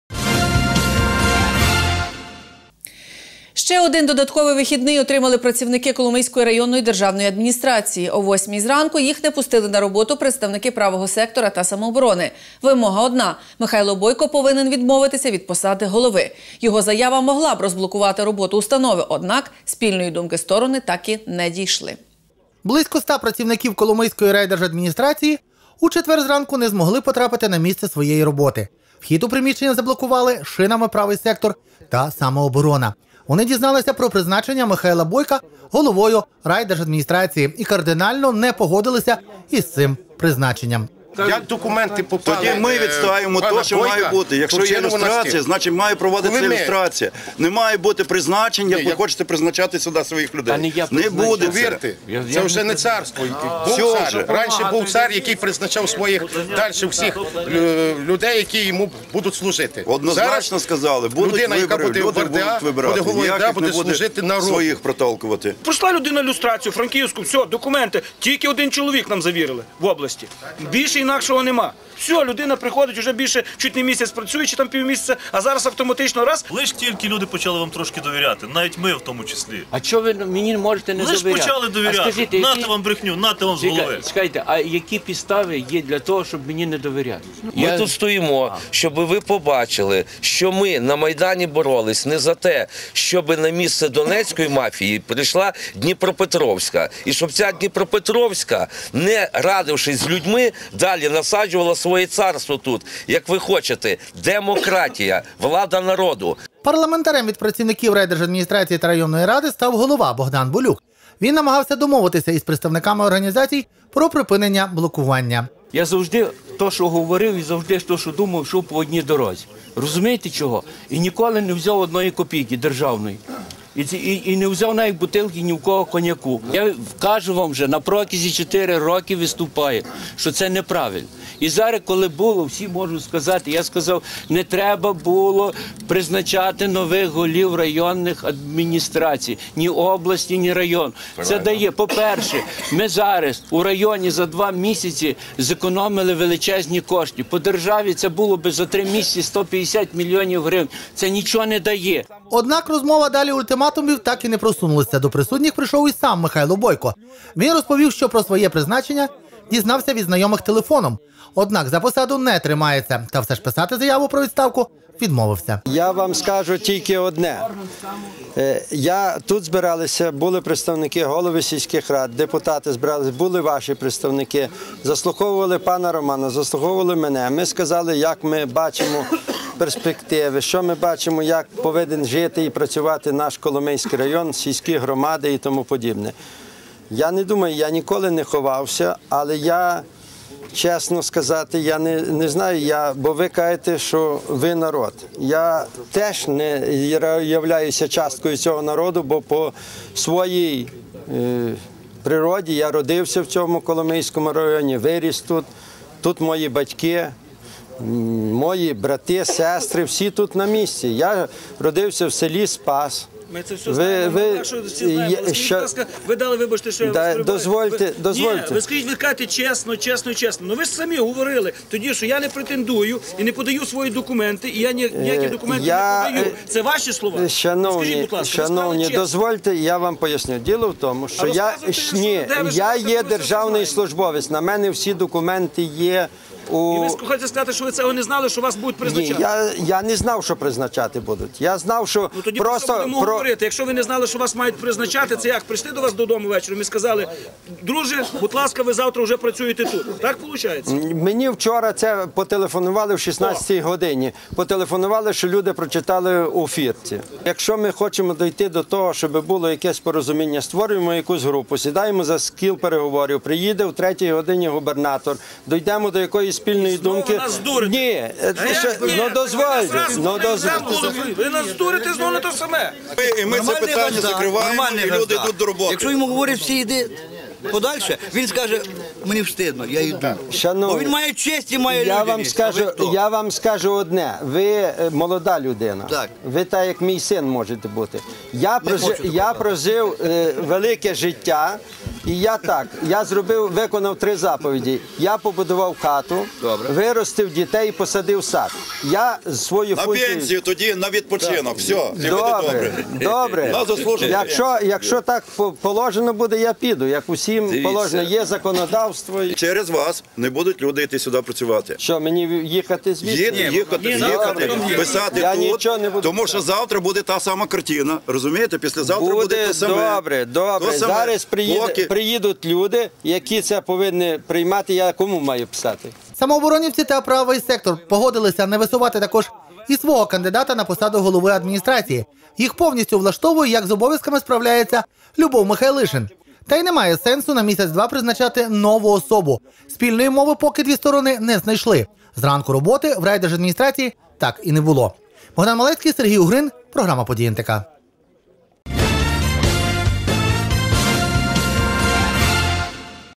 Працівників Коломийської РДА не пустили на роботу
Ще один додатковий вихідний отримали працівники Коломийської районної державної адміністрації. Їх не пустили на роботу представники Правого сектора та Самооборони. Вимога одна -- Михайло Бойко повинен відмовитися від посади голови. Його заява могла б розблокувати роботу установи. Однак, спільної думки сторони так і не дійшли.